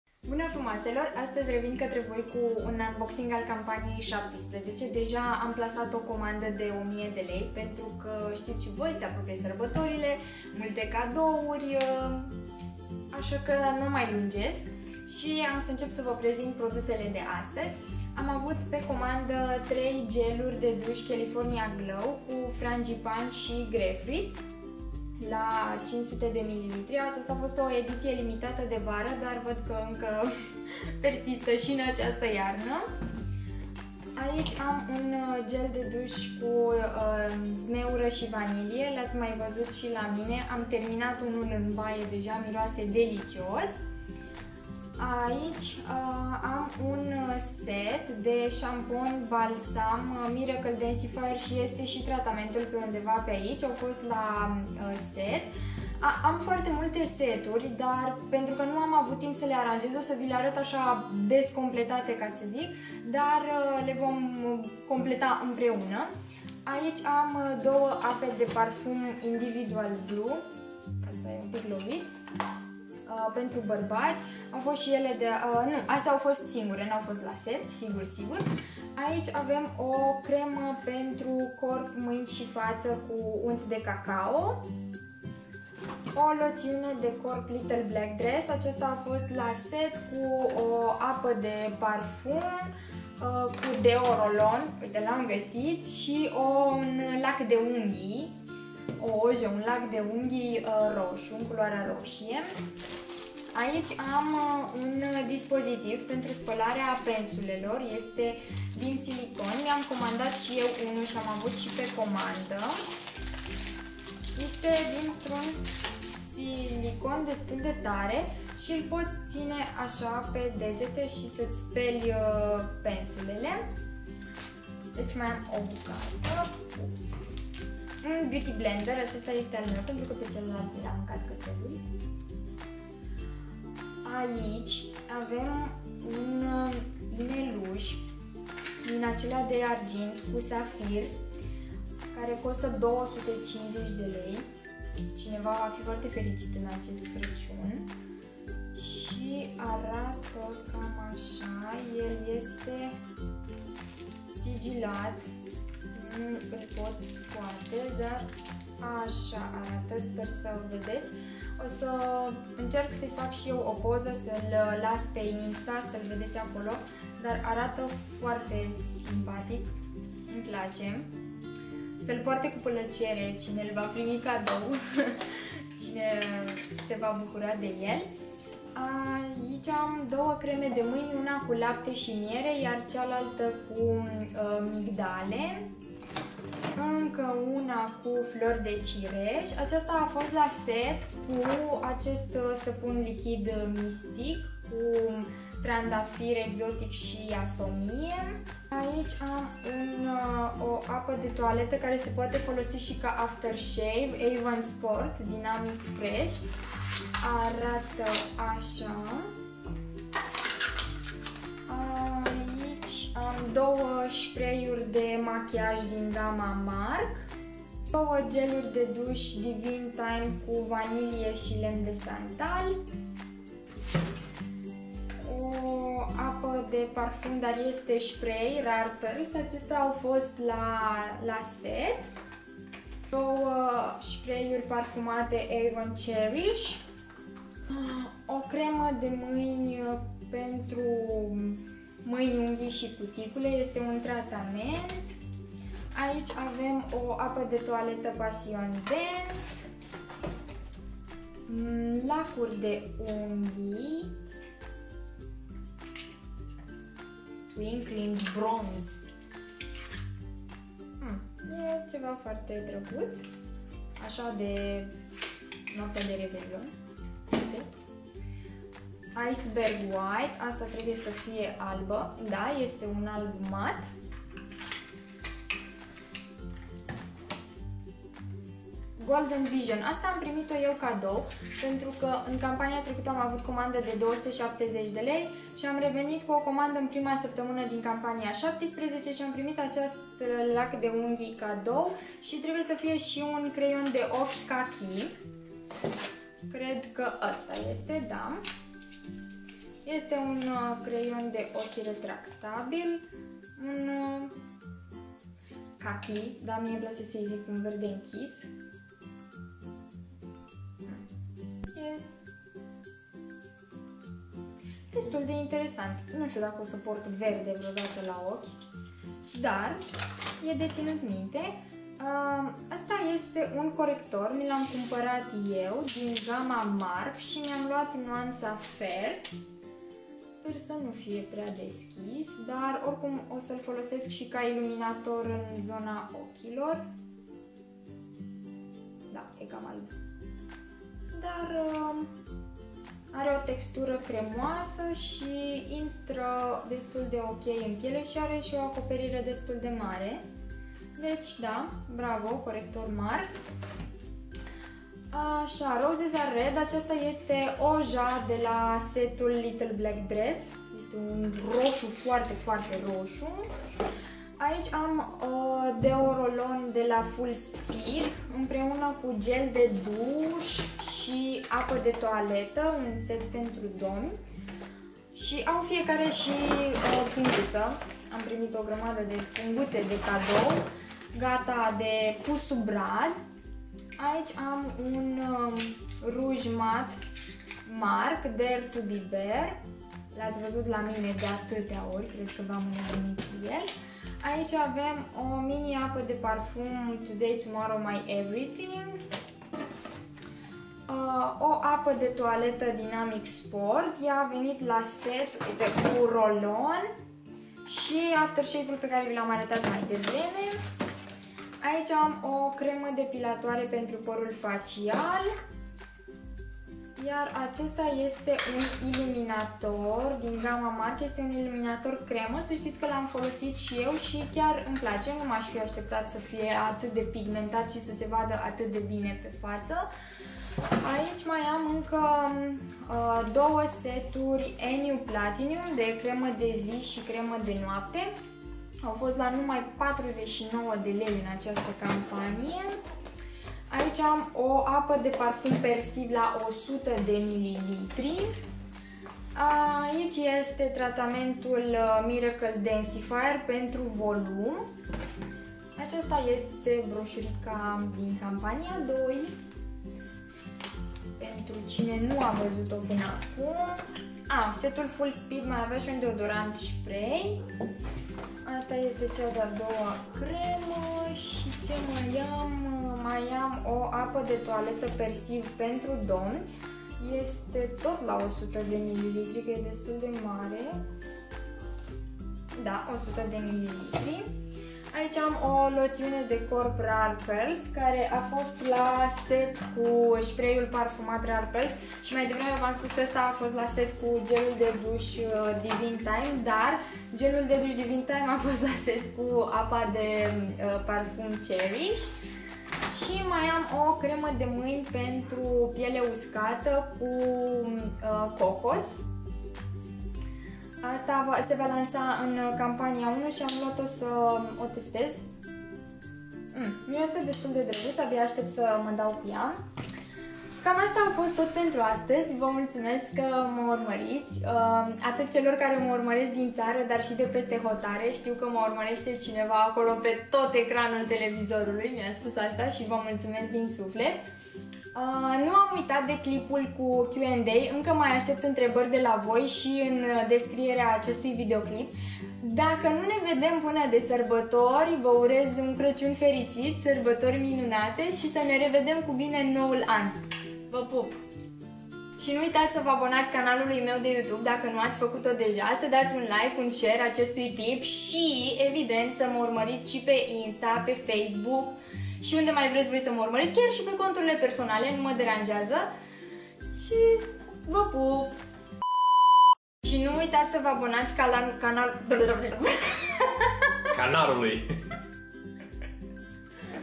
AVON | Unboxing C17/2017 | Primul video cu noua camera
Hei, buna tuturor,

Mi-am cumparat camera, in sfarsit, scuzati luminozitatea scazuta, softbox-urile inca nu au sosit dar sunt pe drum. Nu uitati sa-mi lasati in mesaje intrebarile voastre pentru Q&A.

Va pupacesc si ne revedm curand la Q&A!
Elena